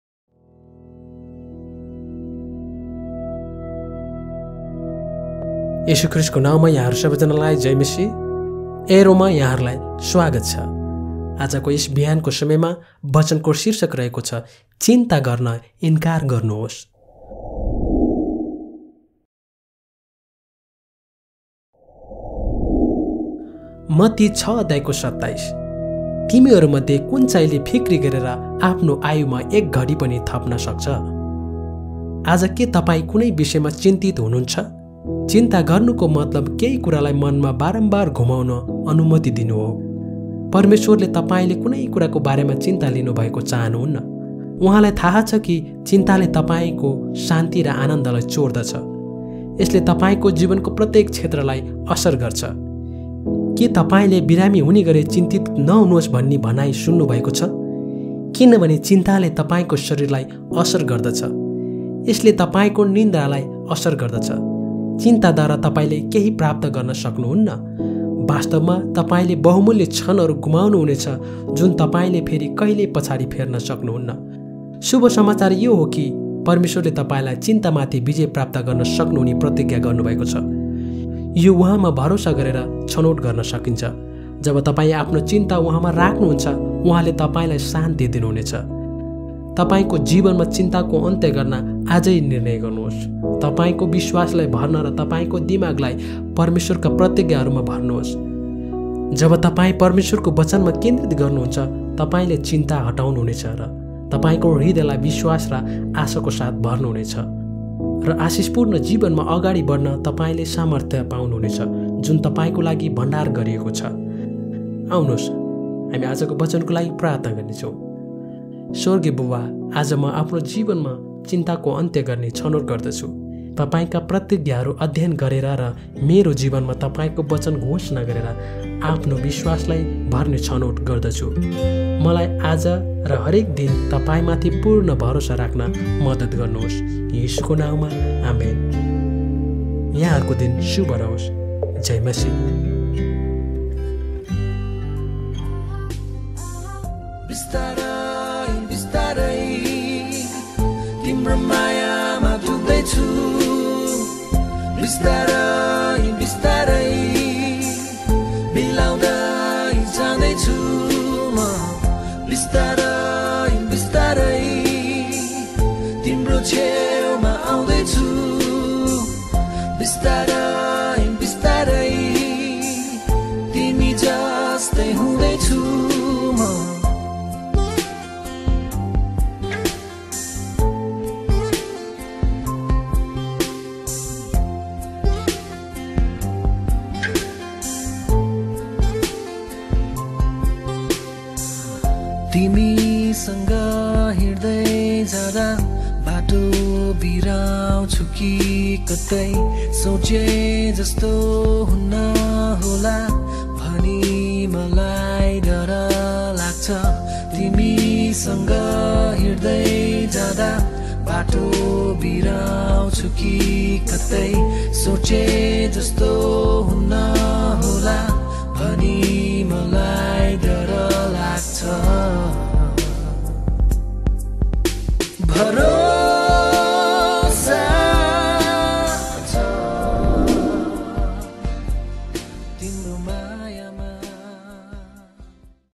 चिन्ता गर्न इन्कार गर्नुहोस् | Reject to worry | 26th September | ARROW
चिन्ता गर्न इन्कार गर्नुहोस्
मत्ती ६ :२७
तिमीहरूमध्ये कुनचाहिँले फिक्री गरेर आफ्नो आयुमा एक घड़ी पनि थप्न सक्छ?
आज के तपाई कुनै विषयमा चिन्तित हुनुहुन्छ ? चिन्ता गर्नुको मतलब, केहि कुरालाई मनमा बारम्बार घुमाउन अनुमति दिनु हो। परमेश्वरले तपाईले कुनै कुराको बारेमा चिन्ता लिनुभएको चाहनुहुन्न। उहाँलाई थाहा छ कि चिन्ताले तपाईंको शान्ति र आनन्दलाई चोर्दछ। यसले तपाईंको जीवनको प्रत्येक क्षेत्रलाई असर गर्छ। के तपाईंले "बिरामी हुने गरि चिन्तित नहुनुहोस्" भन्ने भनाइ सुन्नुभएको छ? किनभने चिन्ताले तपाईंको शरीरलाई असर गर्दछ। यसले तपाईंको निद्रामा असर गर्छ। चिन्ता द्वारा तपाईले केहि प्राप्त गर्न सक्नुहुन्न; वास्तवमा तपाईले बहुमूल्य क्षणहरू गुमाउनुहुनेछ जुन तपाईले फेरि कहिले पछाडि फेर्न सक्नुहुन्न। शुभ समाचार यो हो कि परमेश्वरले तपाईंलाई चिन्तामाथि विजय प्राप्त गर्न सक्नु हुने प्रतिज्ञा गर्नुभएको छ। यो उहाँमा भरोसा गर्न छनौट गरेर शुरू हुन्छ। जब तपाईं आफ्नो चिन्ता उहाँमा राख्नुहुन्छ, उहाँले तपाईंलाई शान्ति दिनुहुनेछ।
तपाईंको जीवनमा चिन्ताको अन्त्य गर्न आज निर्णय गर्नुहोस्। तपाईंको विश्वासलाई भर्न र तपाईंको दिमागलाई परमेश्वरका प्रतिज्ञाहरूमा भर्नुहोस्। जब तपाईं परमेश्वरको वचनमा केन्द्रित ध्यान गर्नुहुन्छ, तपाईंले चिन्ता हटाउनुहुनेछ र तपाईंको हृदयलाई विश्वास र आशाको साथ भर्नुहुनेछ; र आशिषपूर्ण जीवनमा अगाडि बढ्न तपाईं सामर्थ्य हुनुहुनेछ जुन तपाईंको लागि भण्डार गरिएको छ !
आजको लागि प्रार्थना
"स्वर्गीय बुबा, आज म आफ्नो जीवनमा चिन्ताको अन्त्य गर्ने छनौट गर्दछु। तपाईका प्रतिज्ञाहरू अध्ययन गरेर र मेरो जीवनमा तपाईंको वचन घोषणा गरेर आफ्नो विश्वासलाई भर्ने छनौट गर्दछु। मलाई आज र हरेक दिन तपाईमाथि पूर्ण भरोसा राख्न मद्दत गर्नुहोस। येशूको नाममा, आमेन। "
प्रभुमा